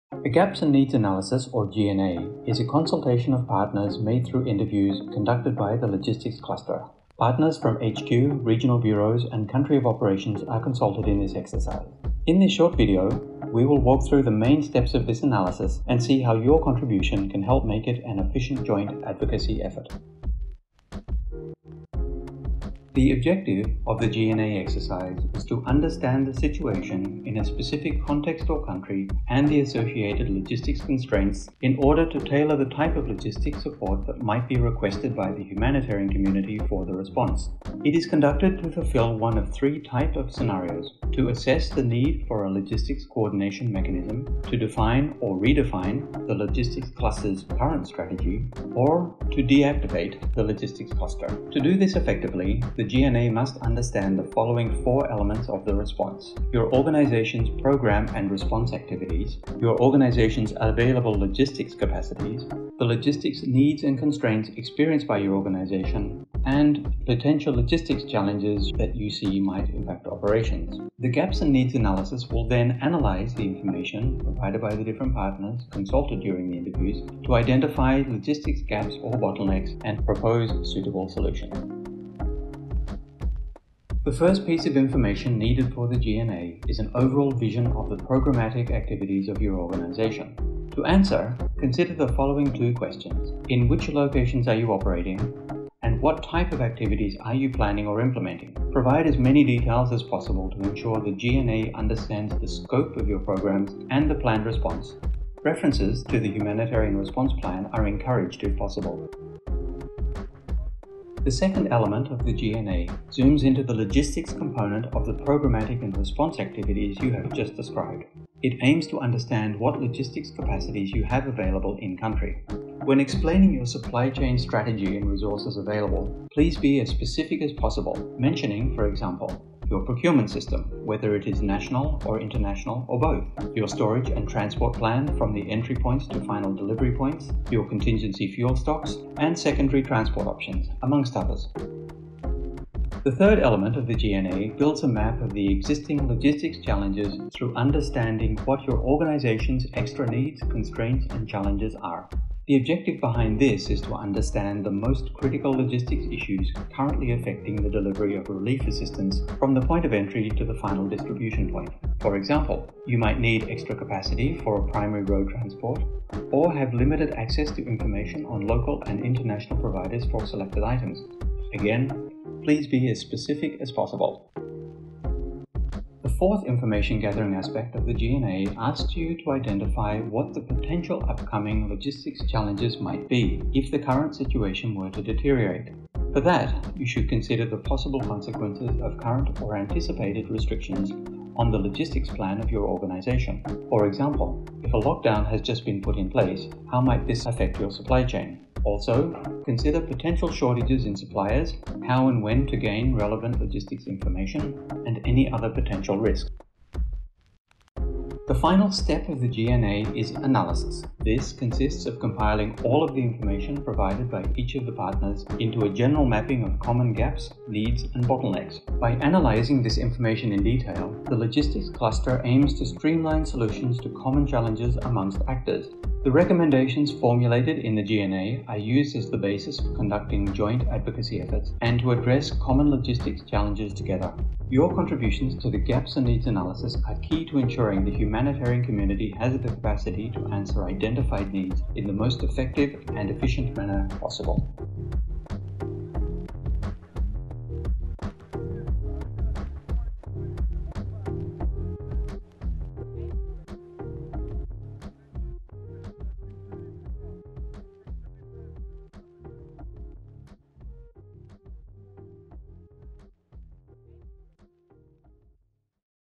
The Gaps and Needs Analysis Exercise
What is a Gaps and Needs Analysis Exercise? And how can the contribution of your organisation help make it an efficient advocacy effort for logistics in humanitarian operations?
In this short video, learn about the main steps of this exercise and the information you should provide to ensure the humanitarian community has the capacity to answer to identified logistics gaps in the most effective and efficient  manner.